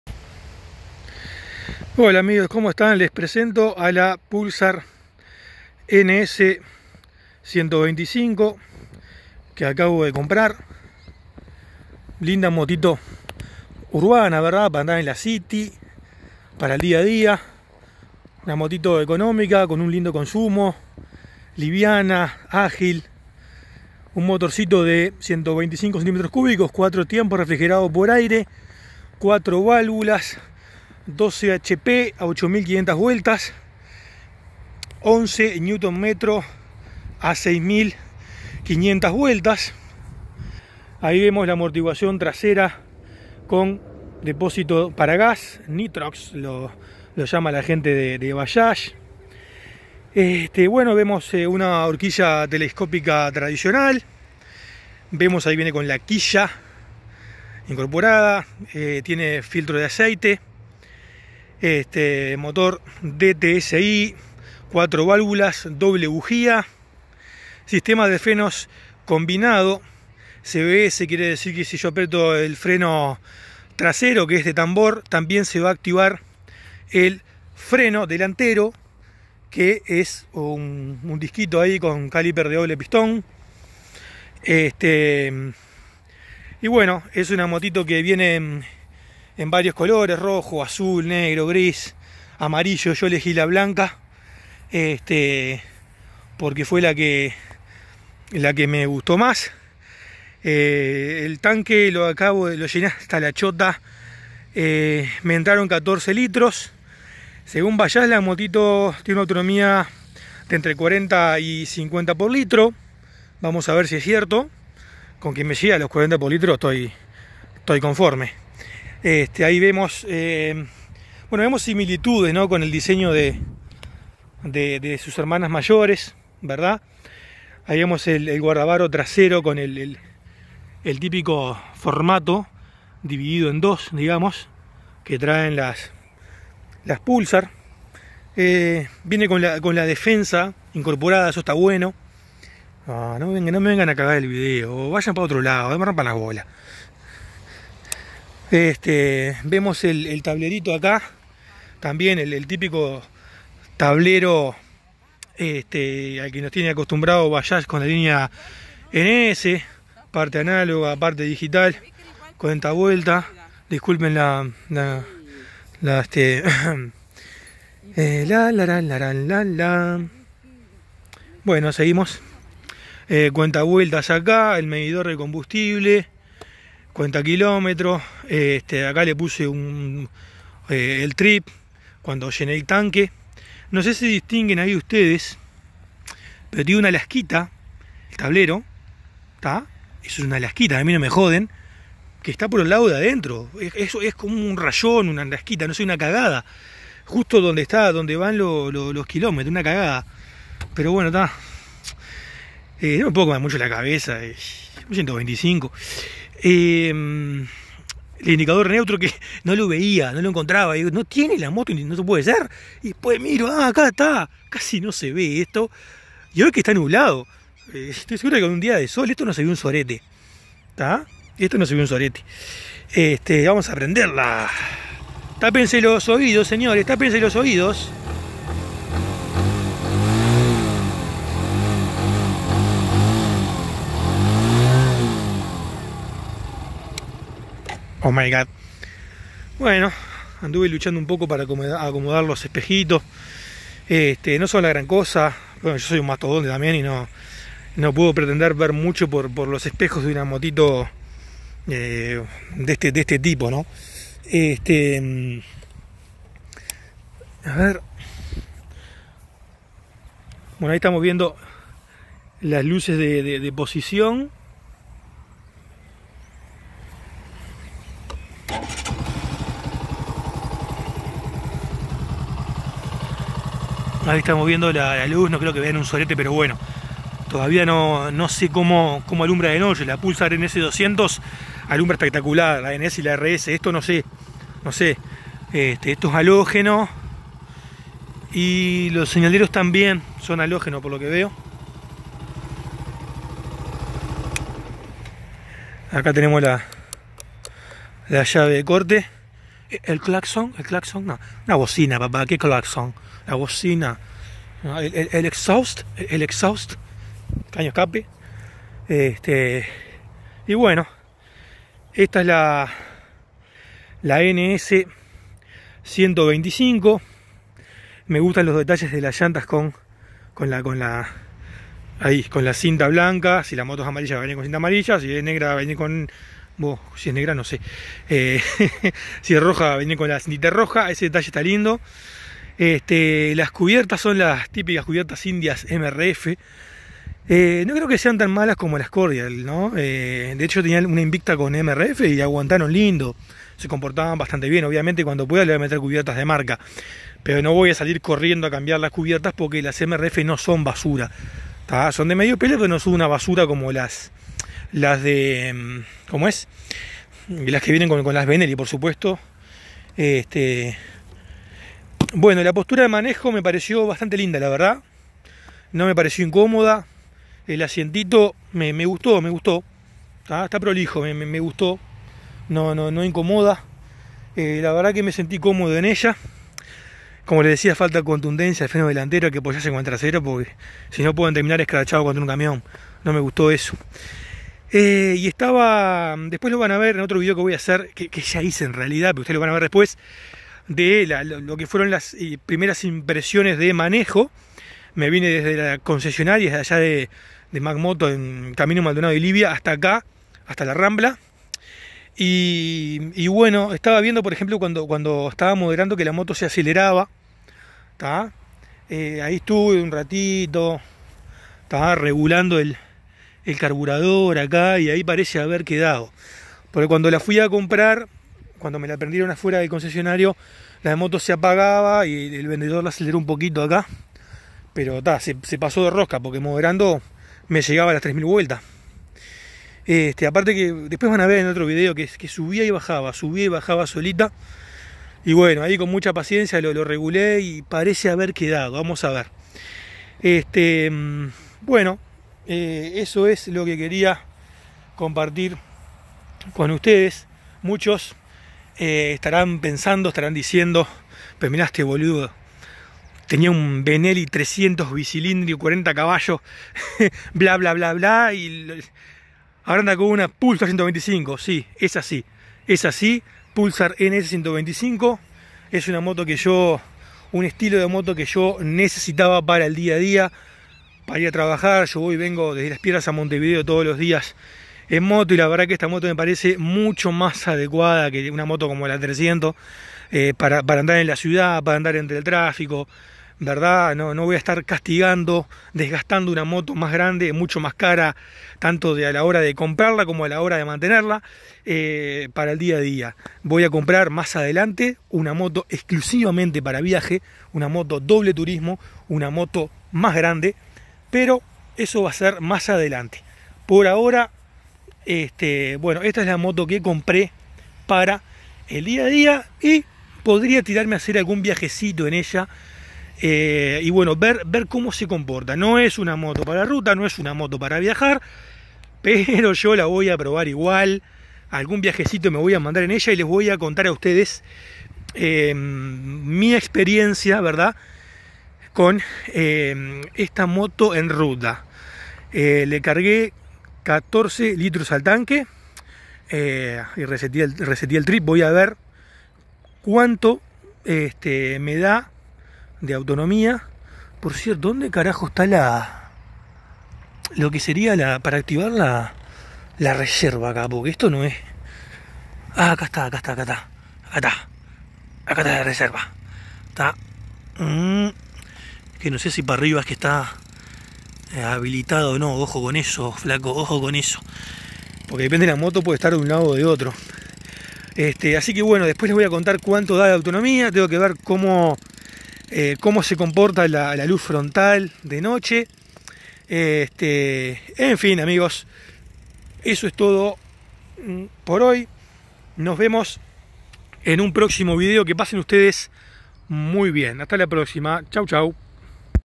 Bajaj Pulsar NS 125 - Todos los Detalles.
Les cuento las especificaciones técnicas y todos los detalles del último modelo de bajaj que llegó al país: la ns 125.